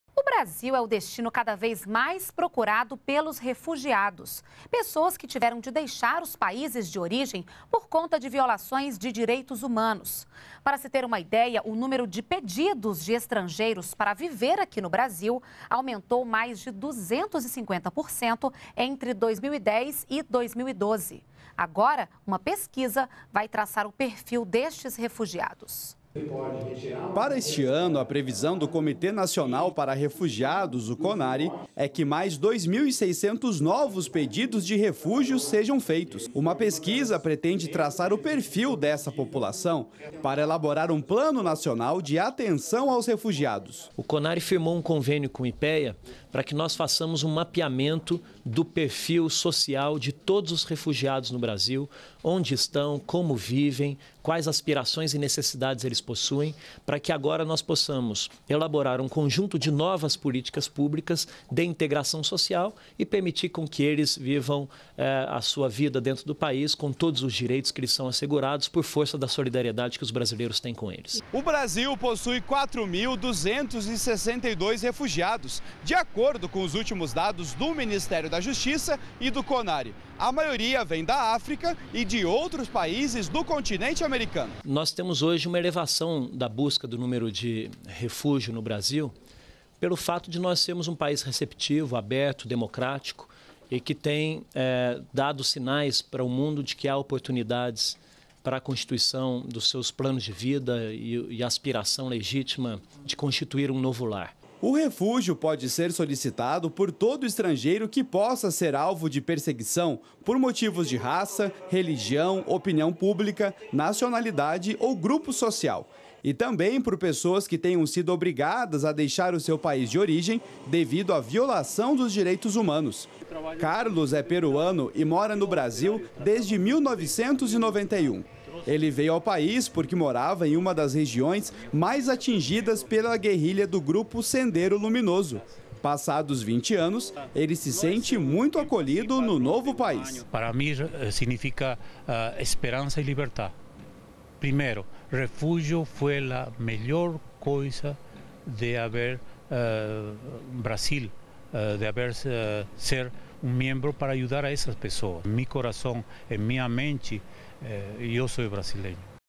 Pesquisa vai traçar perfil de refugiados em território brasileiro
NBR NOTÍCIAS - 03.06.13: O Brasil é um destino cada vez mais procurado por refugiados. Entre 2010 e 2012, o número de pedidos de estrangeiros para viver no Brasil aumentou mais de 250%. O refúgio pode ser solicitado por todo estrangeiro que possa ser alvo de perseguição por motivos de raça, religião, opinião pública, nacionalidade ou grupo social. E também, por pessoas que tenham sido obrigadas a deixar seu país de origem devido à violação de direitos humanos. Segundo o Ministério da Justiça, existem 4.262 refugiados no Brasil. Para saber quem são, em que condições chegam, de onde mais vêm estrangeiros refugiados; e porque vêm, o Ministério da Justiça e a Agência da ONU para Refugiados firmaram convênio com o IPEA. O objetivo é ter conhecimento suficiente para se criar políticas públicas adequadas para essas pessoas. Mais informações no